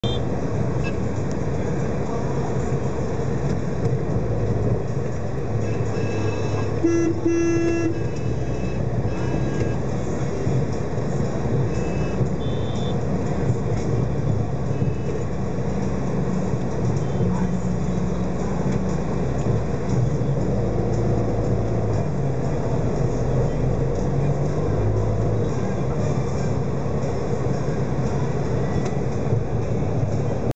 P1010165.MOV
India has many ways of moving people around and in most of the larger cities the traffic can be very chaotic.